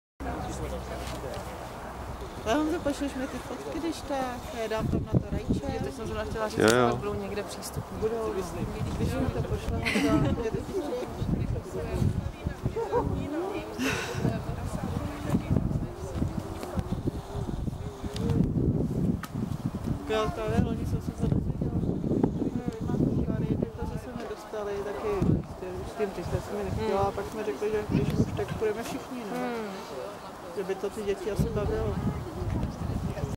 Klokani v Rudné
Výlet s Petrou Ďuranovou za klokany do Rudné